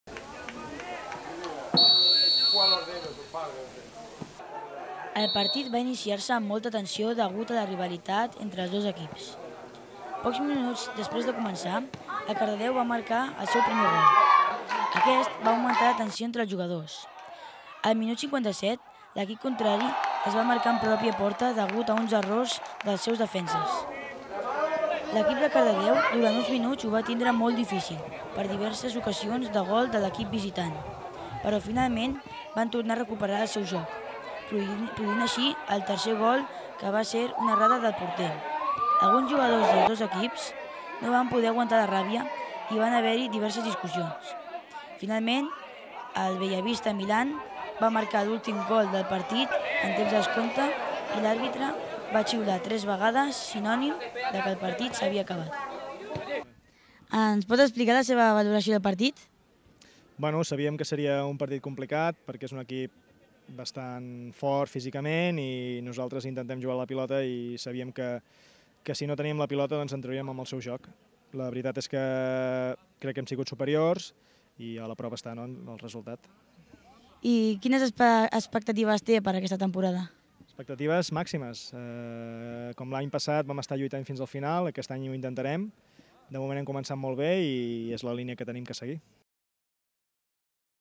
Primera victòria a casa
Obrim plana esportiva amb l'enfrontament del futbol club cardedeu i el bellavista milan al camp municipal d'esports.L'equip local va guanyar per 3-1 al cub futbol bellavista en el que era el primer partit a casa dels cardedeuencs.